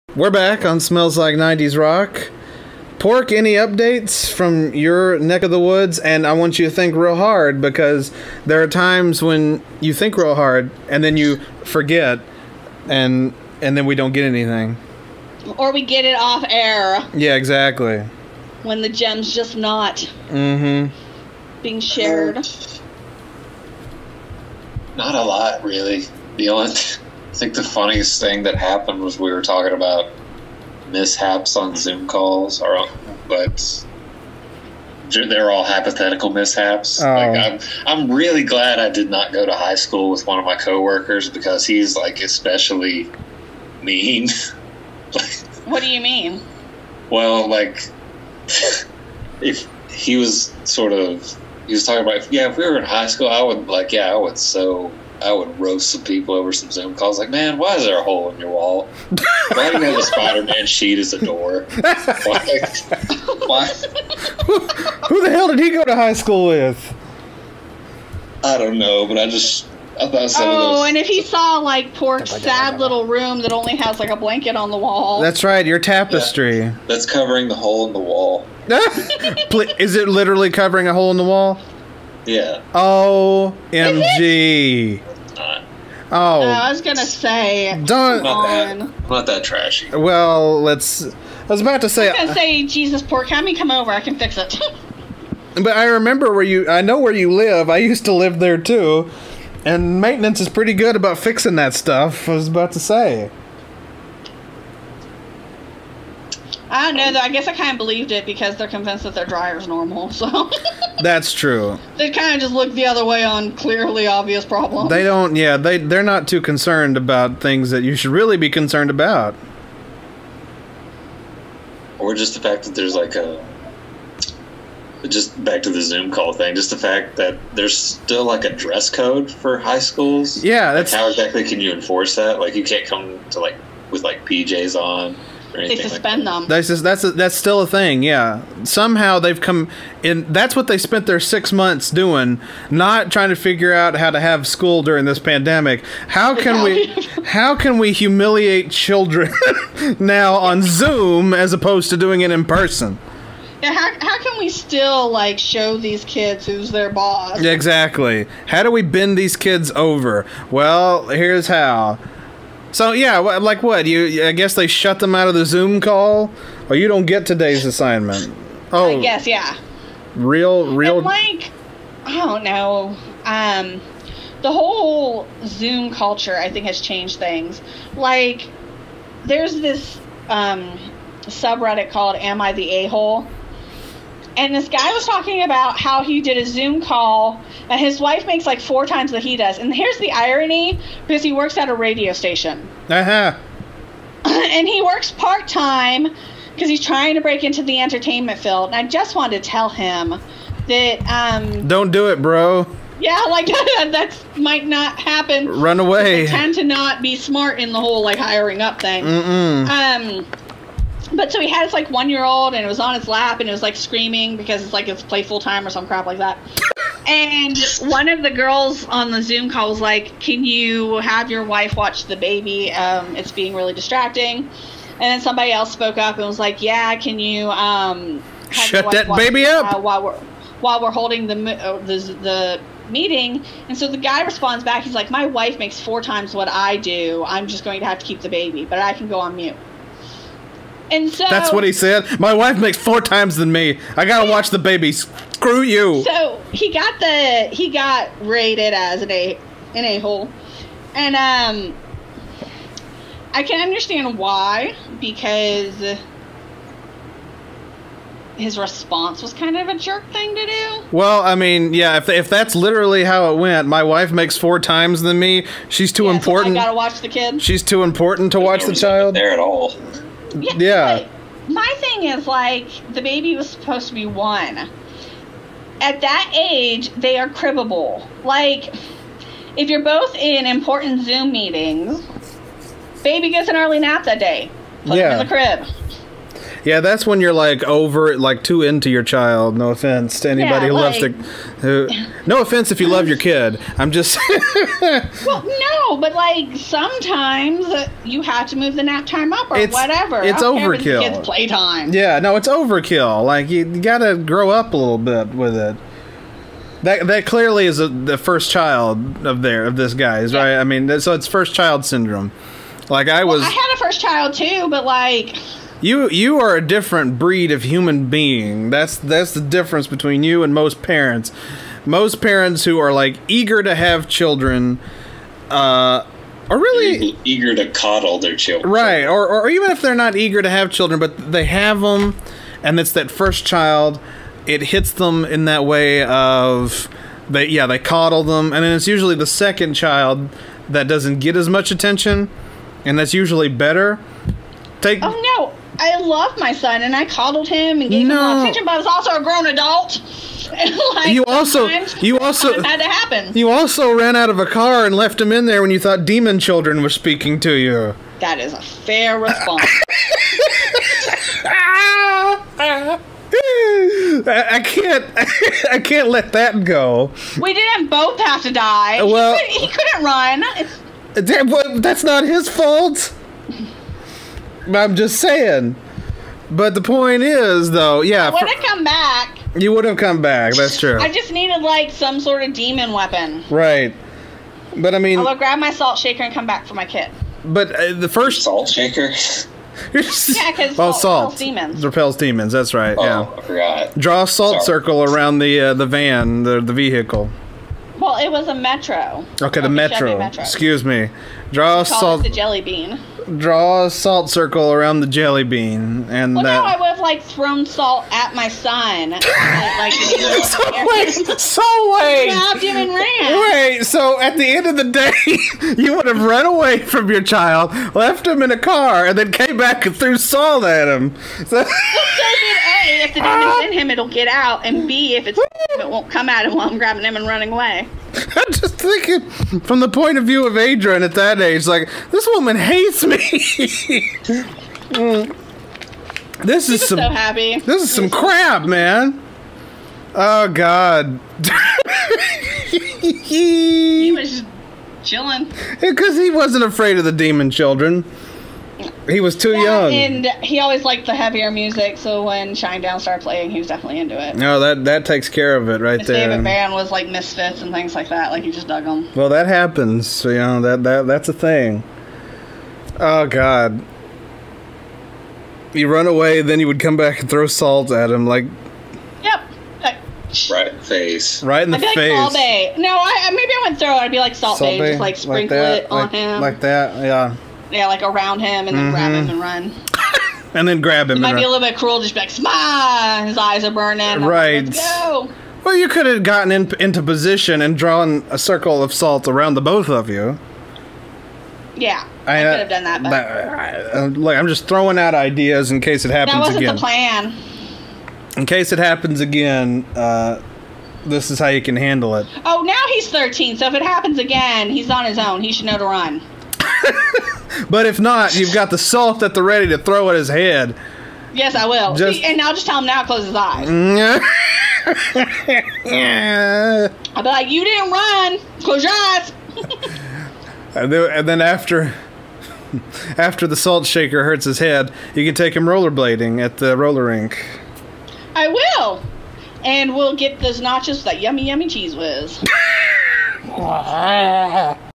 Home Game Round 24: September 12 2020 Segment 1
The gang talks about the strange oddity that is Zoom culture. Then they revisit some of Shiela's strange, but funny stories.